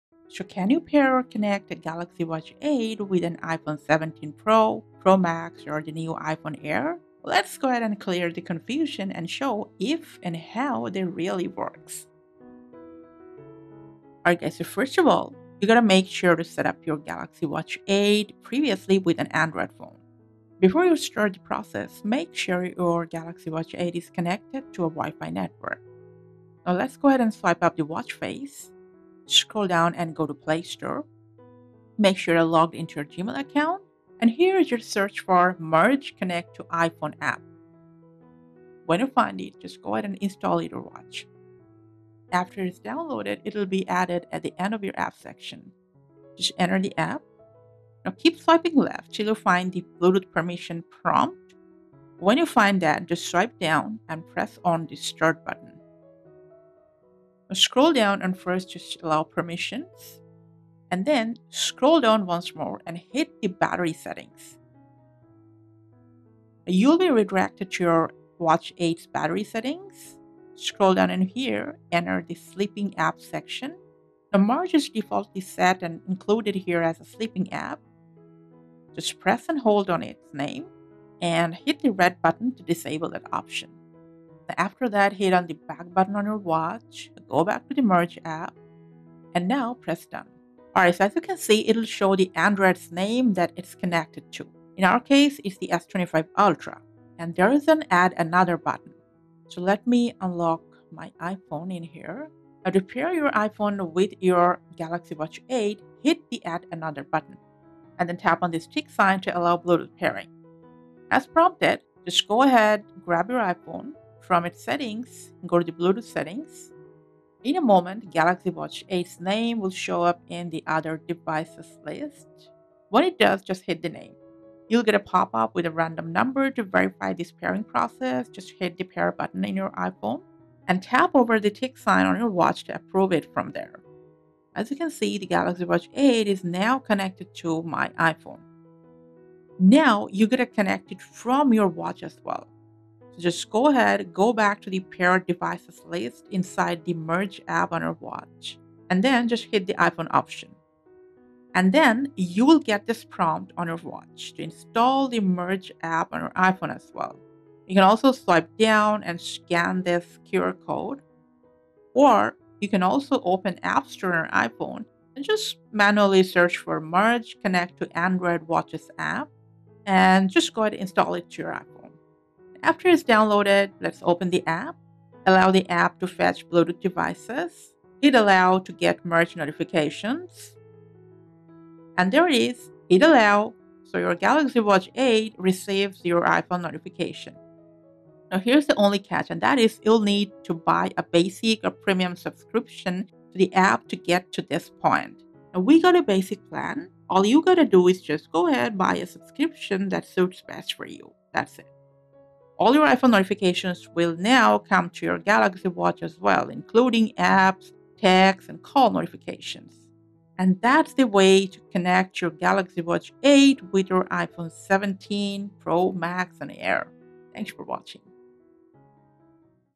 Samsung Galaxy Watch 8: How to Connect to iPhone 17 Pro Max/Air!
Wondering how you can connect your Samsung Galaxy Watch 8 with the latest Wear OS to your iPhone 17 Pro Max or AIR, so you can take calls, text, and Notifications?

Are you looking for a step-by-step guide to pairing your Galaxy Watch 8 to an iPhone 17 Series Phone or any iPhone with the latest iOS as a new user? Let's follow this 3-step beginner's guide to sync and use the Galaxy Watch 8 effortlessly with iPhone 17.

0:00 Intro: Samsung Galaxy Watch 8 Connect to iPhone 17 Pro Max/Air
0:13 Prerequisite to Connect Galaxy Watch 8 to iPhone
0:27 Step 1: Install and Set Up the Connection App on Galaxy Watch
1:32 Step 2: Pair Galaxy Watch 8 to iPhone 17 together
2:32 Step 3: Install and Set Up the App on iPhone
3:10 Please Note about app Subscription